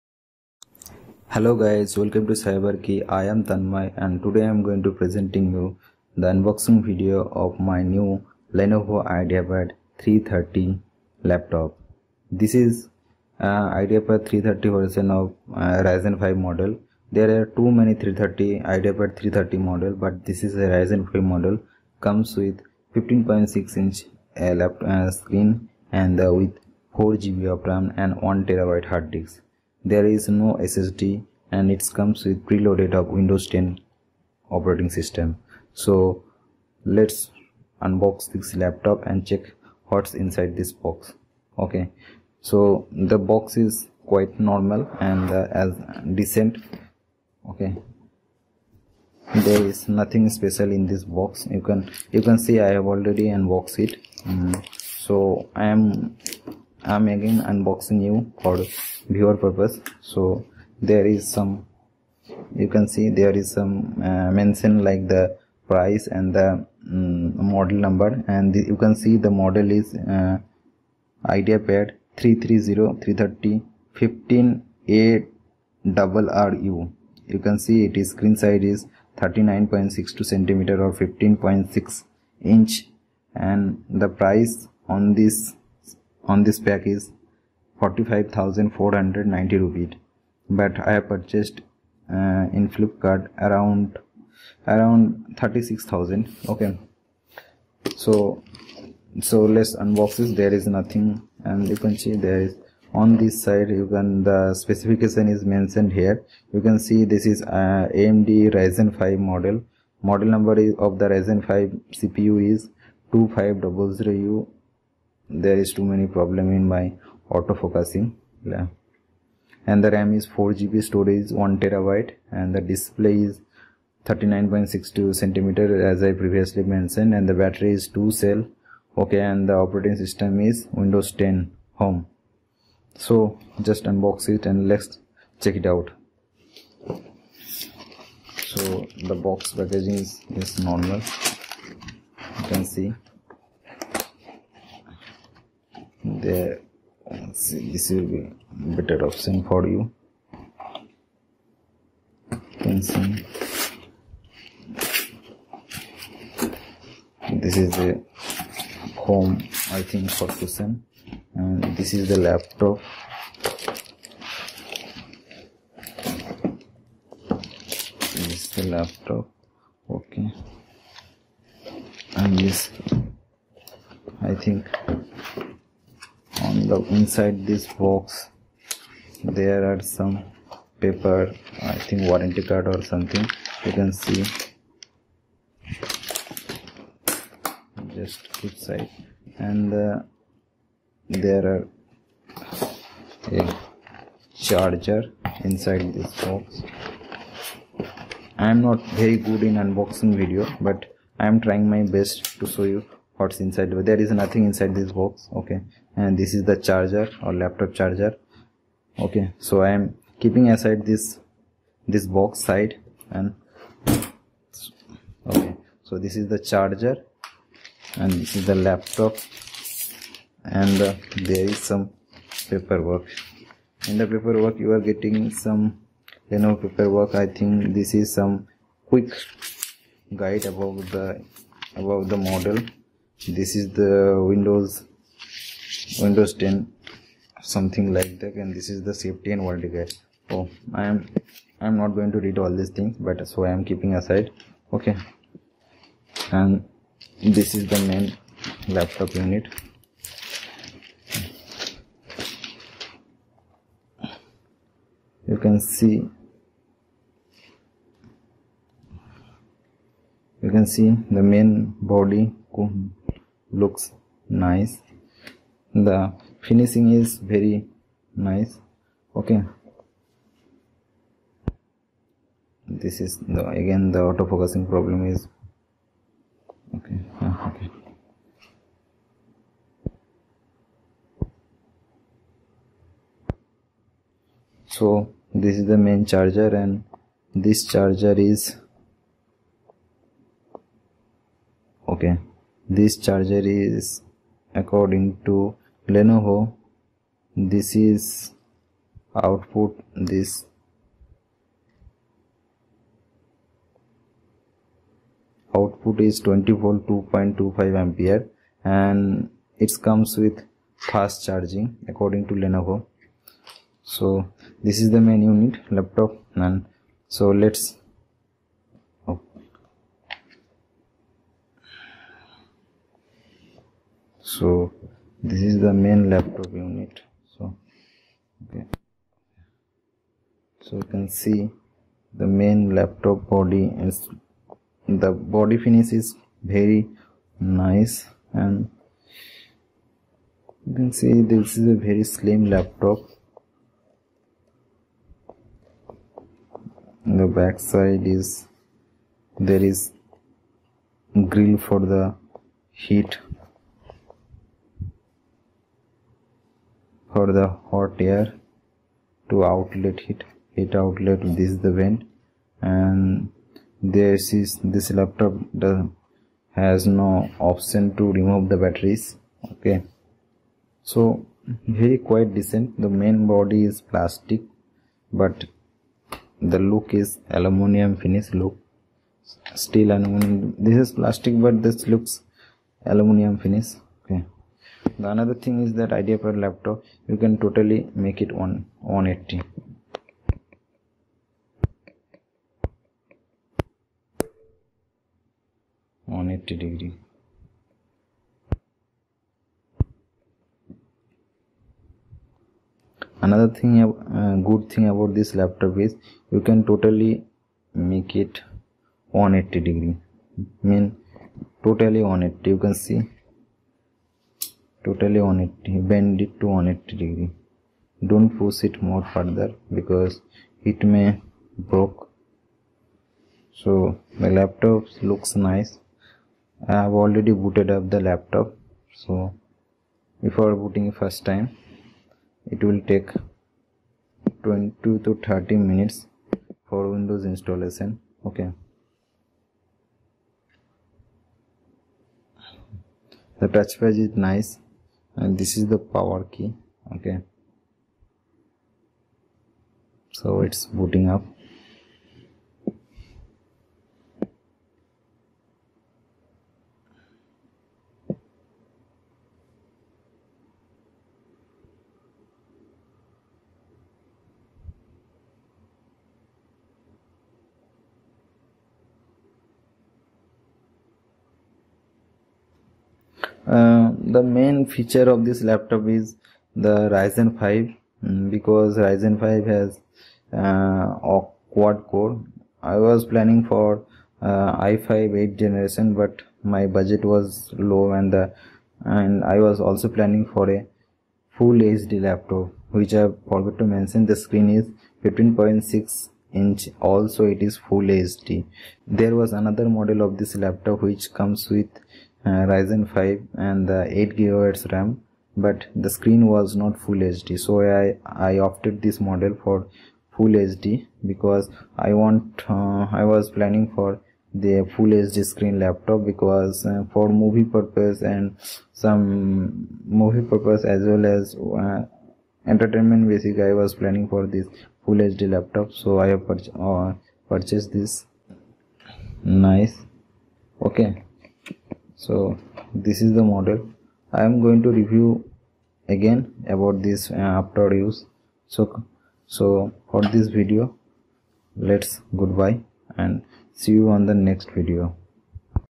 Unboxing Lenovo IdeaPad 330 AMD Ryzen 5
Unboxing Lenovo IdeaPad 330 AMD Ryzen 5  Model Number 15ARR U. 4GB Ram, 1TB hard drive. Vega 8 GPU.

Read my honest review on Lenovo Ideapad 330 AMD Ryzen 5 Laptop